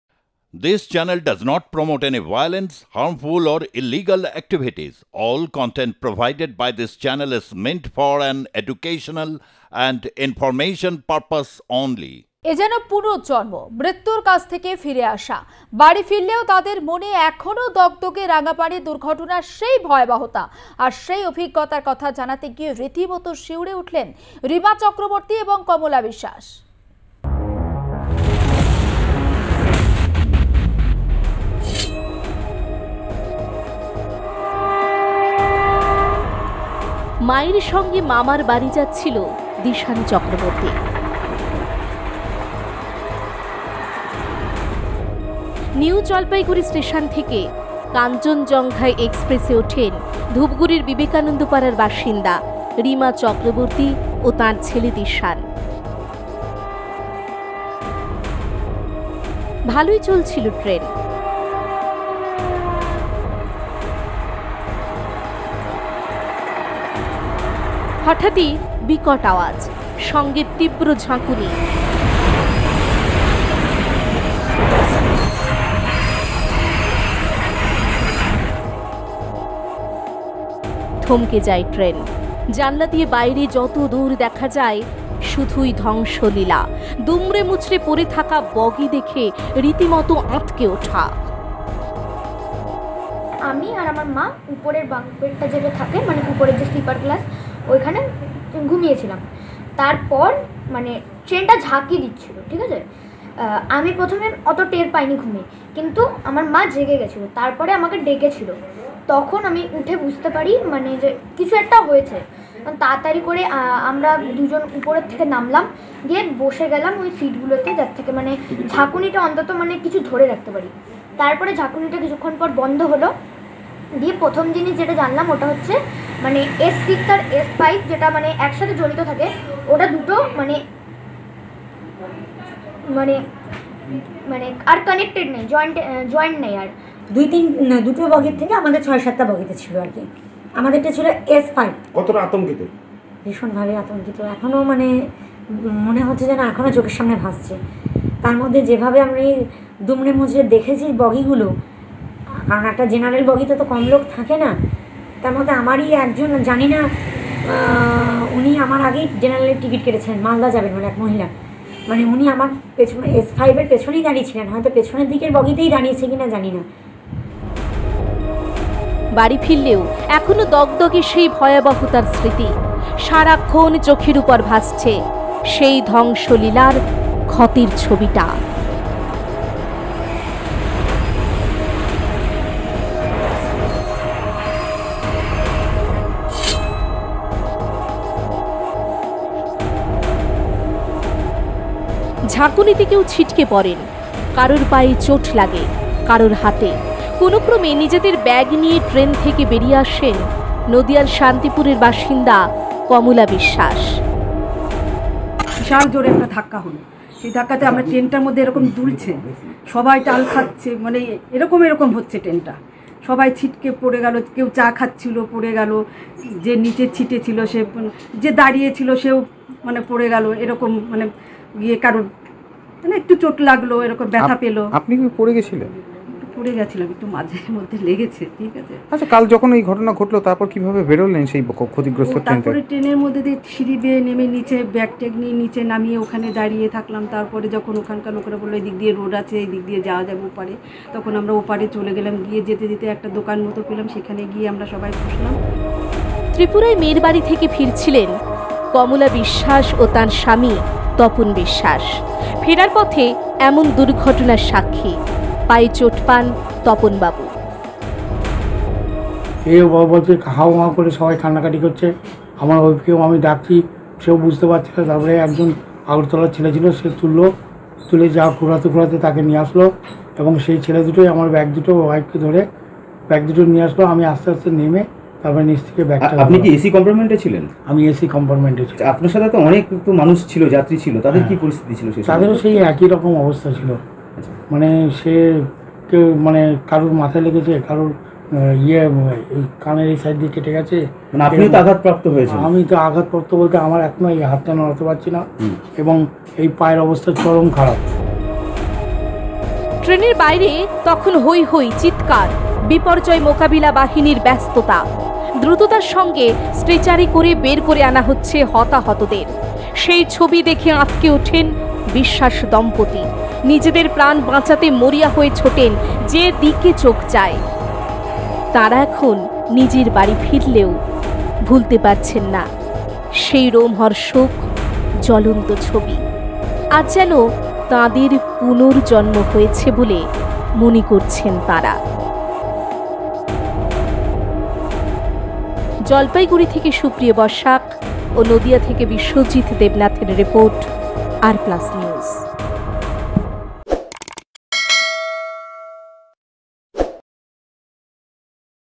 Kanchanjunga Express Accident: রাঙাপানির দগদগে স্মৃতি। R PLUS NEWS
Kanchanjunga Express Accident: মৃত্যুর কাছ থেকে ফিরে এসে এখনও ভাসছে রাঙাপানির দুর্ঘটনার ভয়াবহতার ছবি রিমা চক্রবর্তী এবং কমলা বিশ্বাসের। R PLUS NEWS